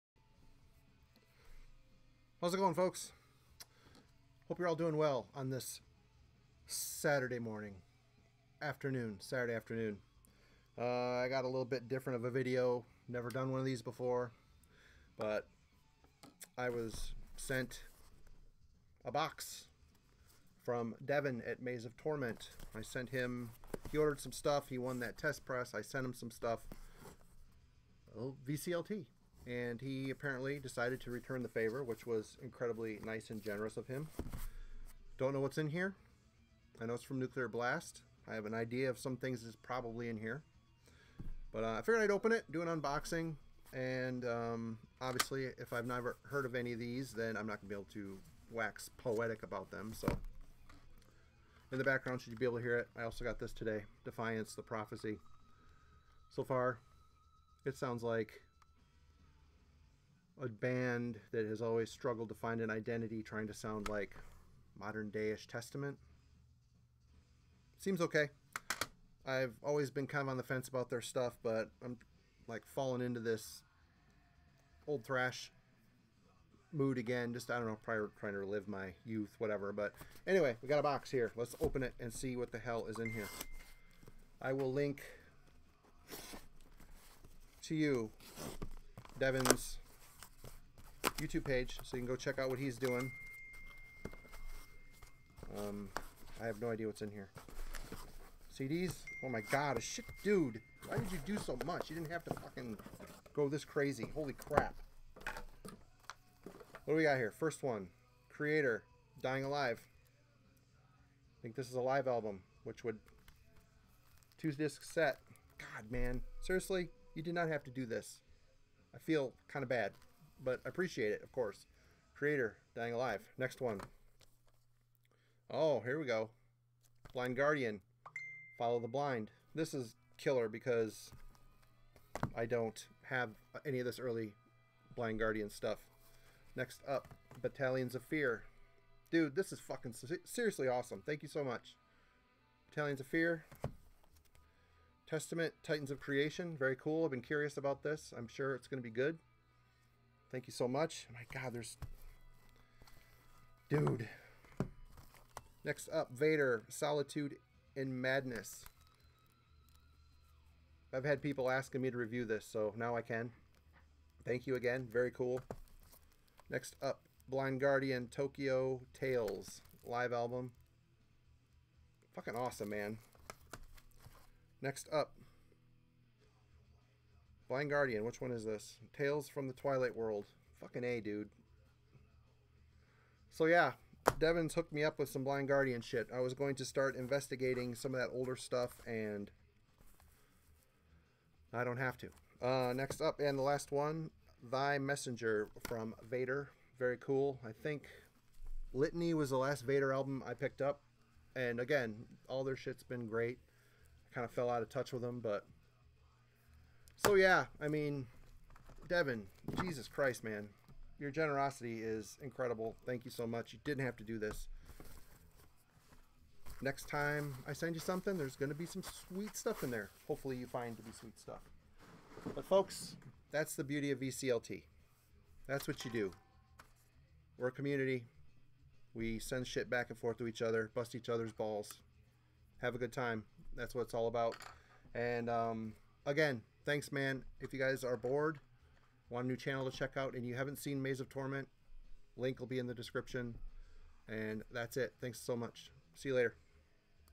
VCLT from Maze of Torment (Devon)!!
He shows some great stuff and is a good dude, hungry to grow his collection and dig for the classics!

Thanks so much Devon!